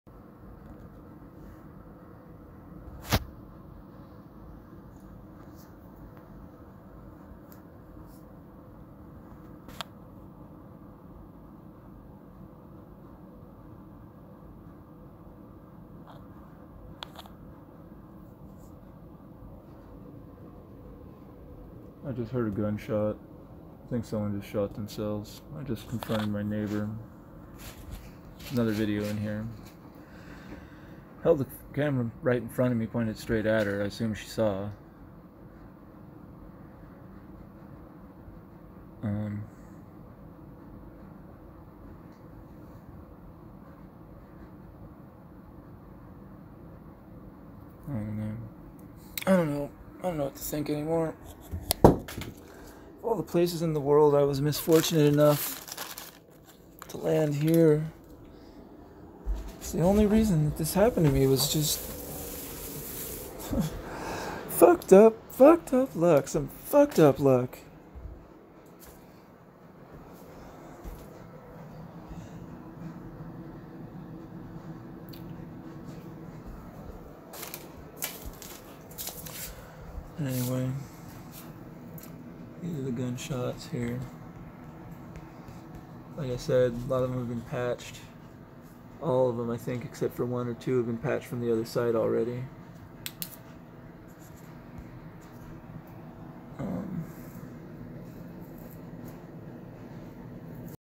Gun shots
Shots fired through my front door. This one hit my foot.  Also holes in the cieling f4om the unit above me.I am last bldg Athens. Next to bridge to park. Bottom back side on the left